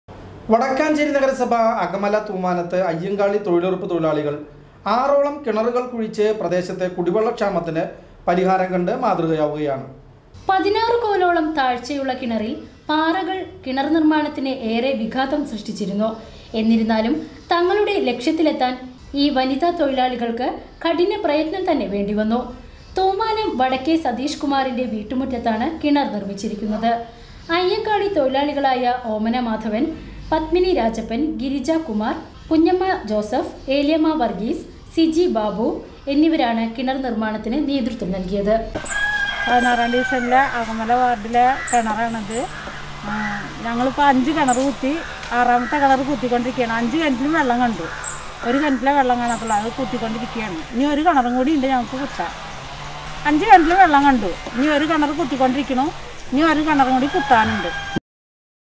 അകമല തൂമനാത്ത് തൊഴിലുറപ്പ് തൊഴിലാളികൾ ആറോളം കിണറുകൾ കുഴിച്ചു
NEWS UPDATES 16-April-2021
VCV NEWS

Wadakkanchery, Thrissur